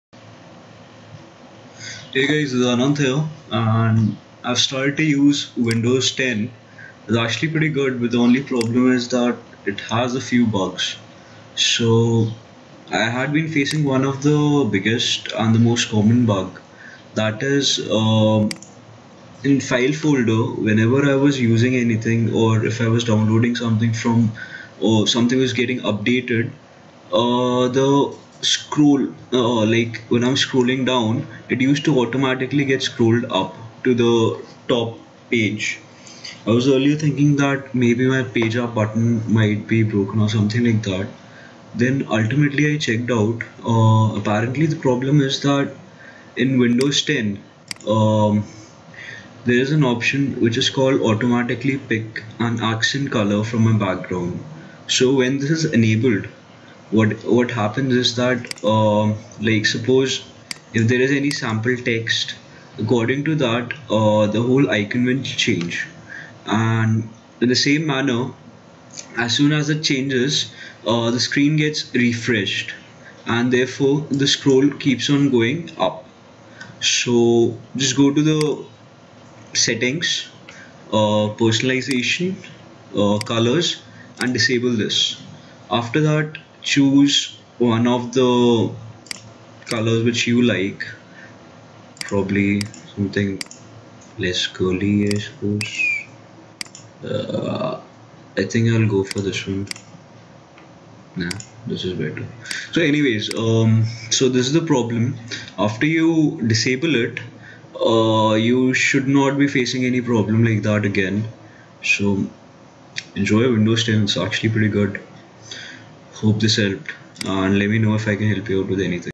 Windows 10 - Automatic scroll up and app popup *Fix*
I just updated to windows 10. The two problems I faced were:
-In file folder no matter how much I scrolled it automatically went to the top again which became frustrating quite fast.
-As samsung was getting the updates the app popup covered my window.

Solution? Go to settings-personalization- Color -Disable automatically pick color accent from my background.
Once that is done it should be fixed. Have fun.